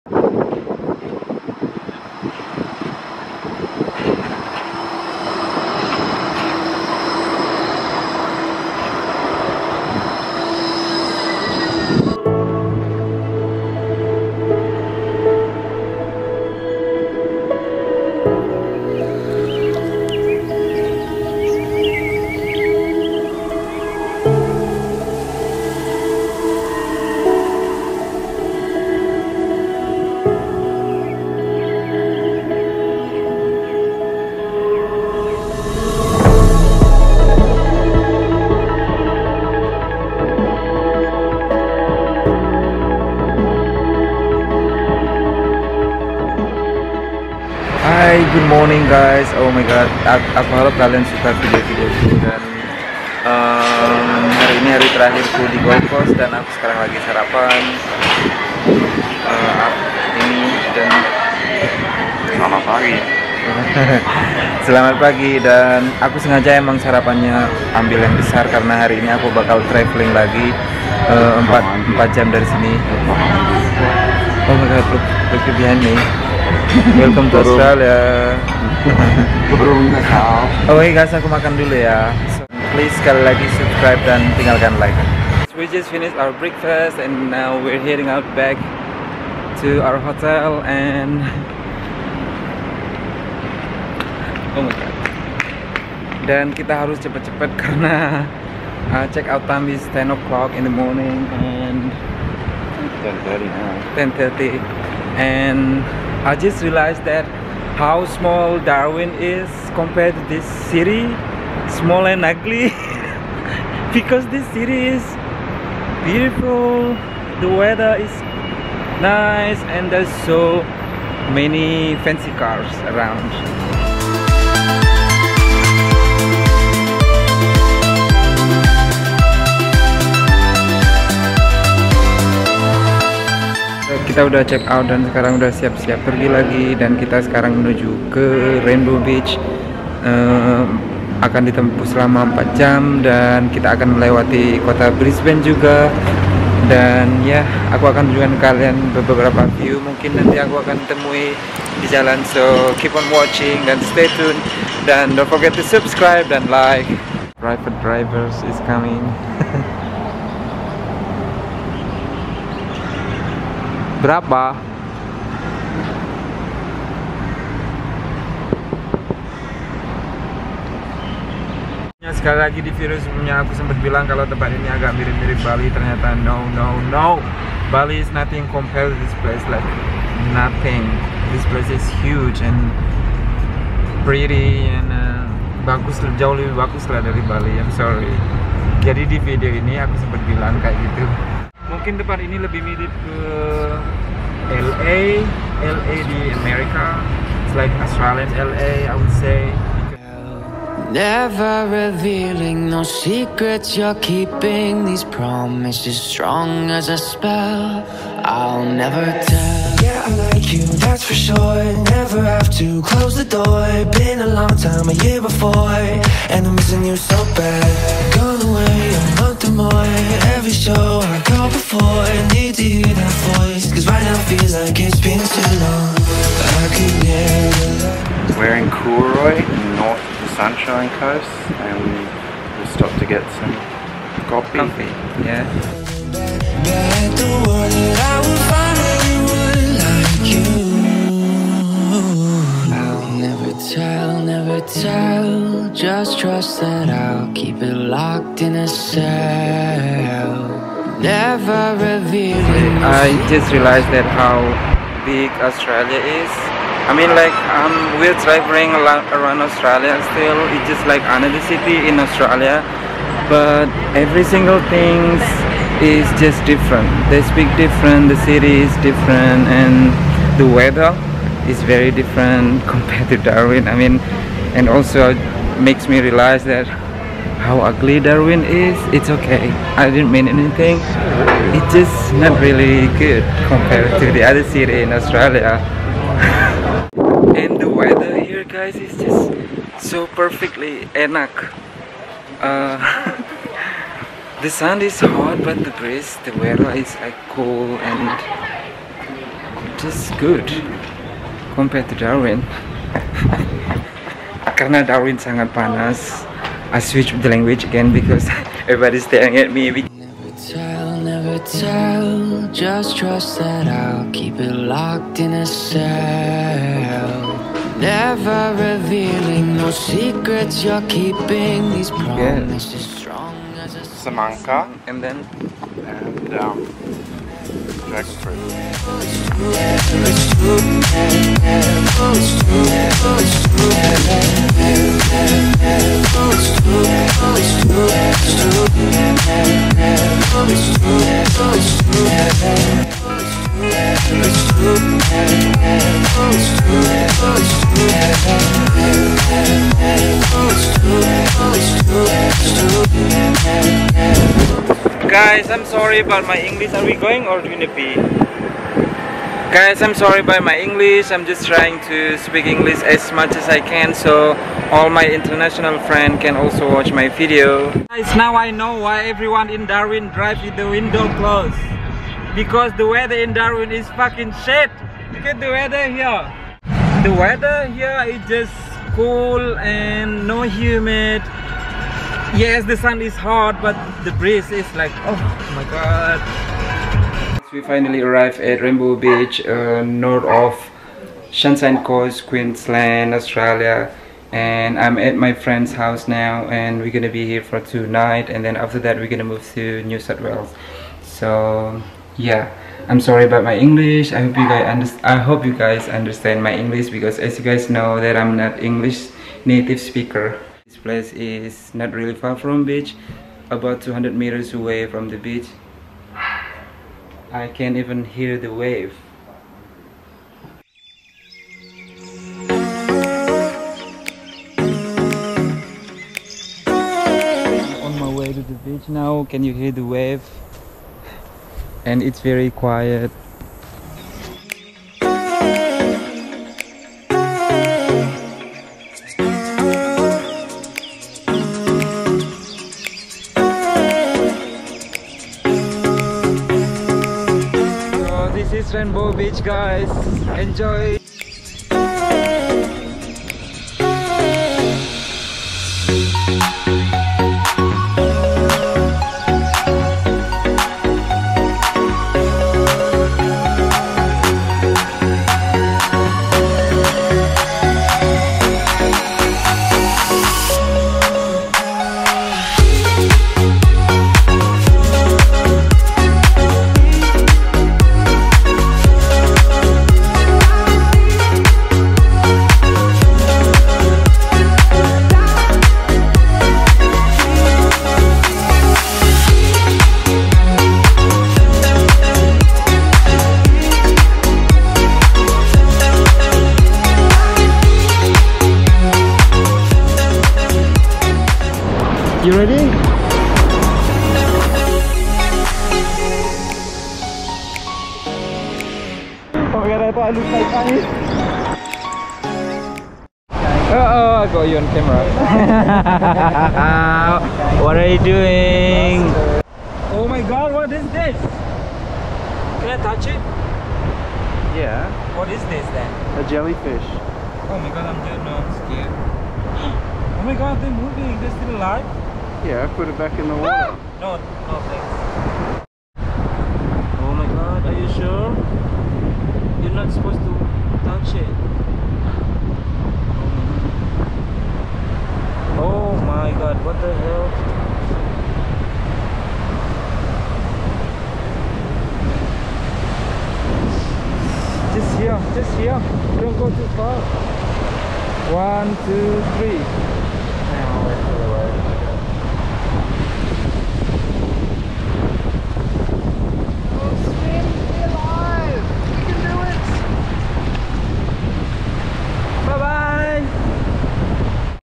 Surfers Paradise Gold Coast ✈️ Rainbow Beach. ll Travelling dimasa pandemi
Traveling dimasa pandemi lumayan berbeda deangn sebelum pandemi. Jadi tonton sampai habis guys kalau kalian ingin gahu gimana prosesnya